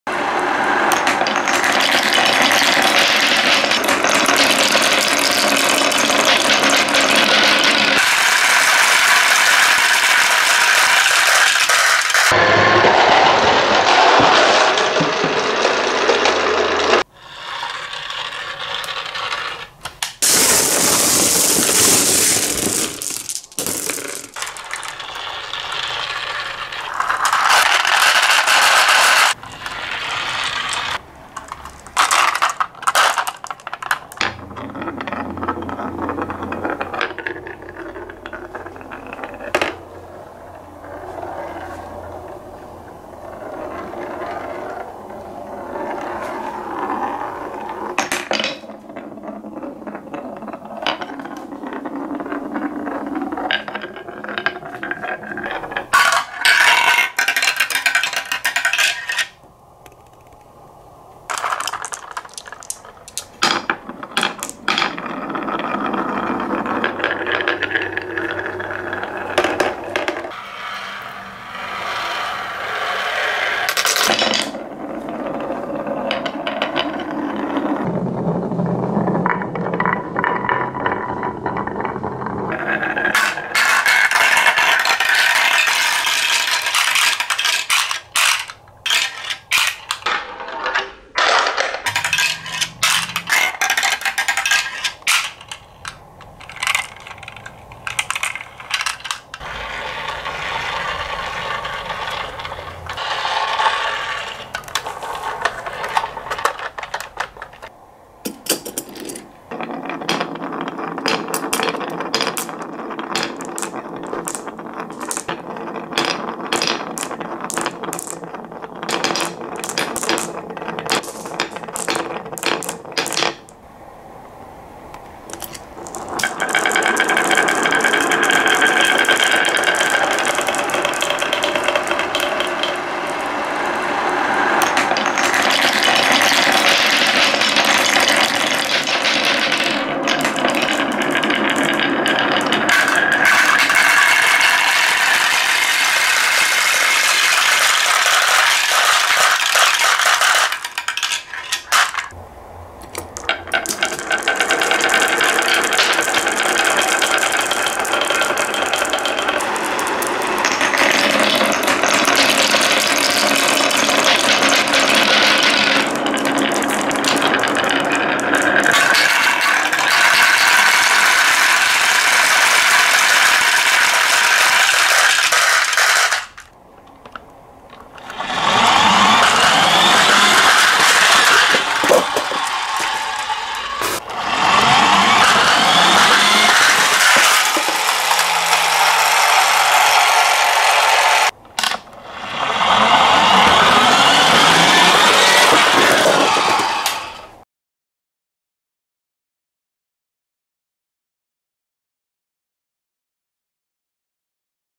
マーブルランレース ASMR ダンプトラック 木製 くみくみスロープ Marble Run Race and Dump Truck Wave Slope
Marble ASMR ビー玉の転がる音が楽しいよ

HABAのウェーブコースをビー玉が走ります。
ビー玉が木製のコースを走る癒しの音を楽しんでください。
Marbles run on the HABA wave course.
Enjoy the healing sound of marbles running on a wooden course.

大人の方がプレゼント購入時などの参考にしていただけたらと思ってます。
※This video is for adults over the age of 14.